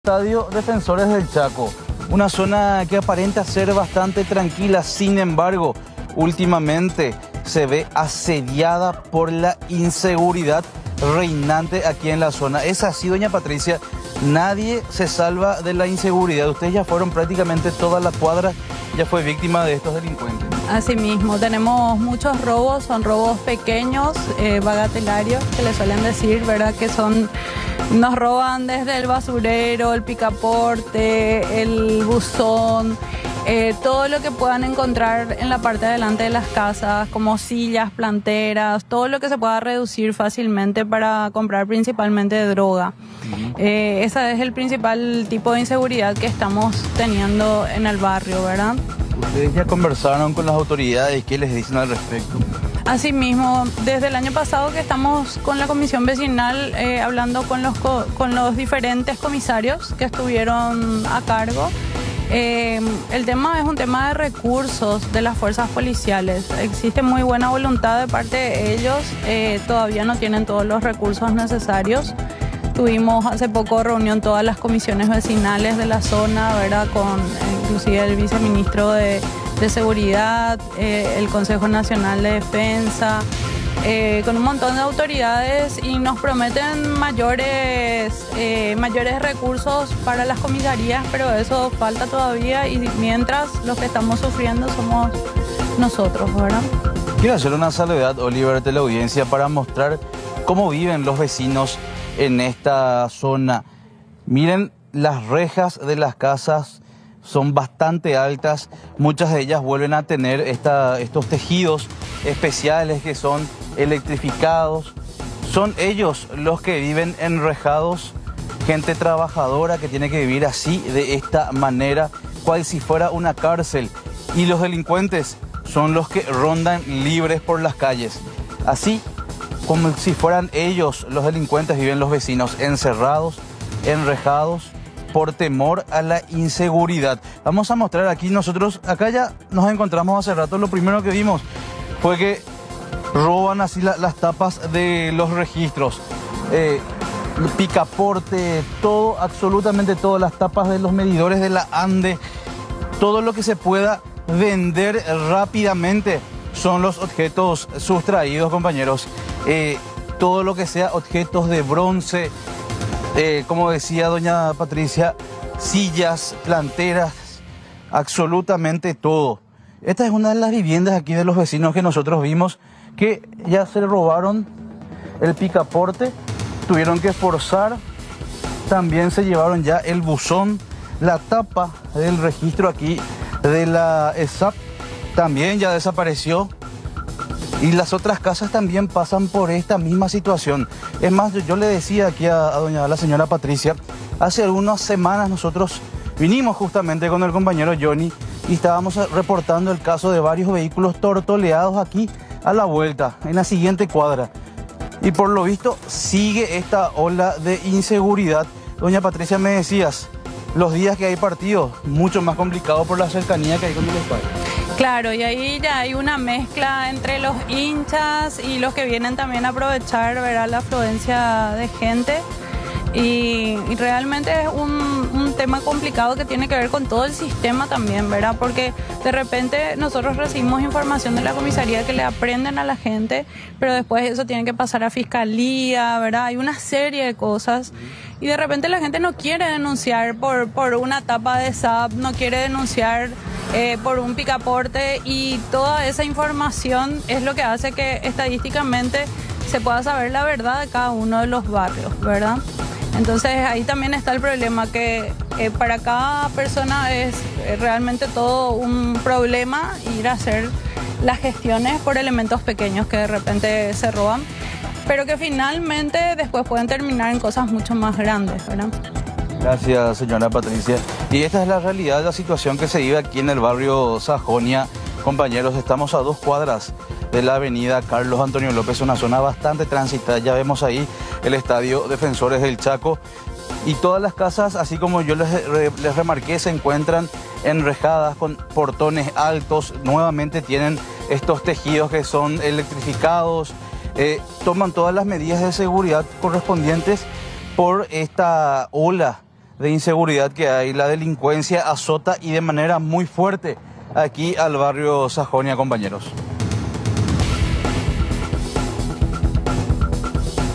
Inseguridad reinante en Sajonia
Un adicto robó focos de una pizzería en el barrio Sajonia de Asunción. La denunciante asegura que son varios los hechos delictivos en la zona